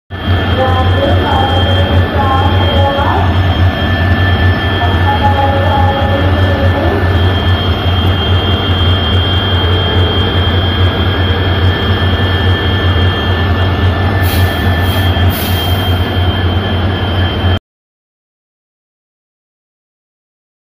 Diesel Fleet At Lucknow Charbagh Jn. WDP 4's & WDP 4D's + Special 40124 Captain Lakshmi Sehgal EMD's
Electro Motive Division Diesel Engine Roaring & Humming sound Diesel Fleet At Lucknow Charbagh Jn. WDP 4's & WDP 4D's + Special 40124 Captain Lakshmi Sehgal EMD's